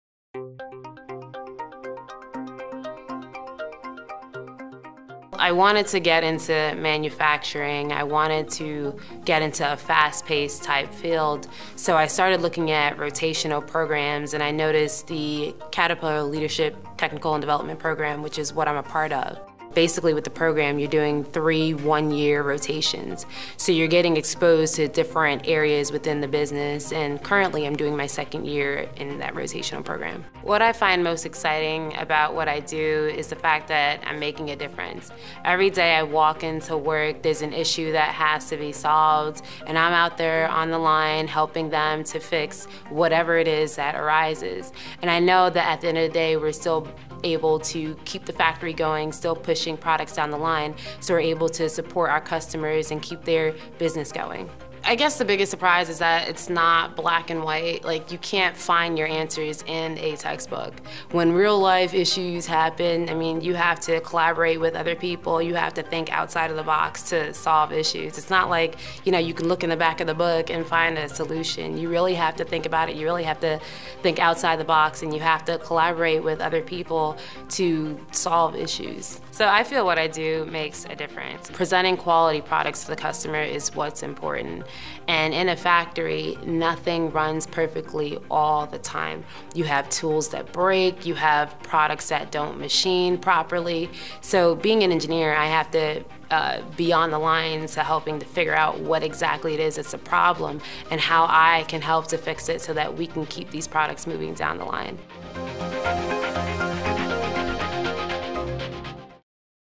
TRMA: Manufacturing Careers--Manufacturing Engineer
Three Rivers Manufacturers' Association -- Manufacturing Careers: Where your ideas become reality. (Caterpillar Manufacturing Engineer)